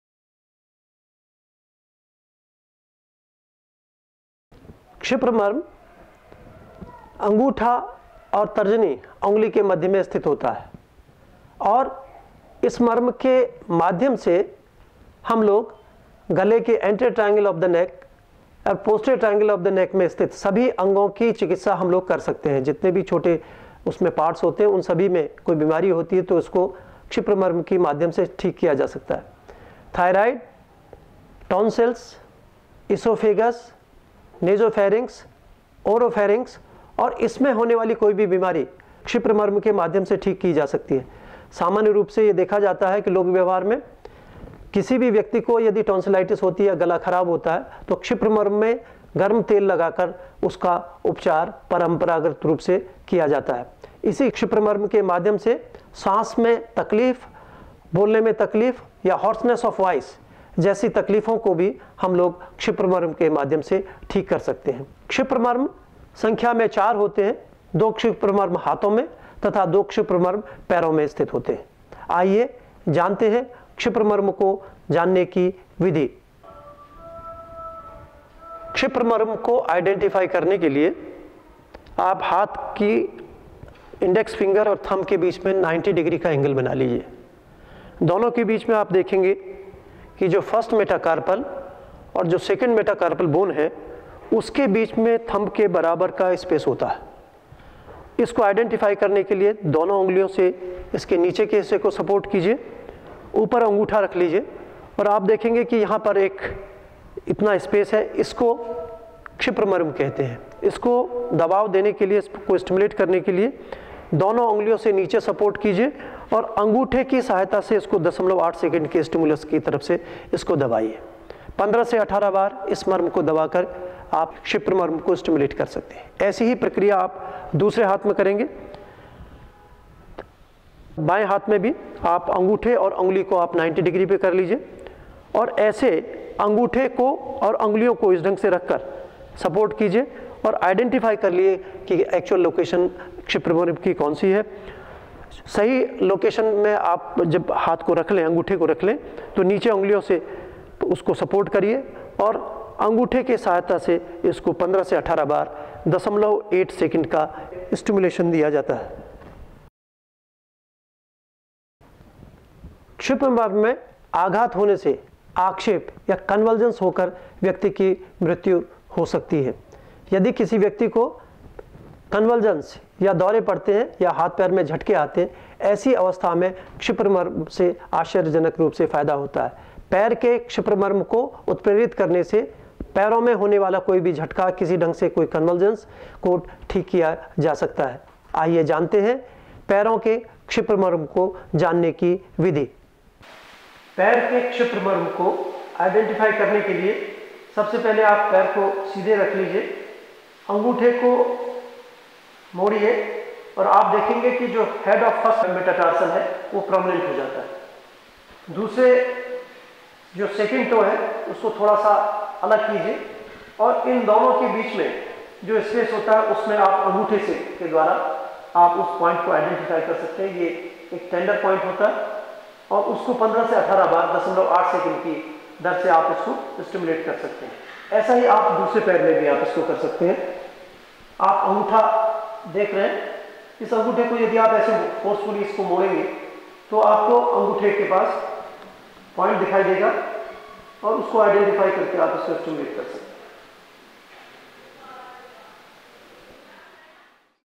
Kshipra marma
Kshipra marma is situated at the webspace of thumb and index finger of both hands as well as legs. By applying pressure about 15-18 times over the kshipra marma you can get relief from the diseases related to neck, thyroiditis, tonsillitis, oesophagitis, pain in legs, convulsions and many more.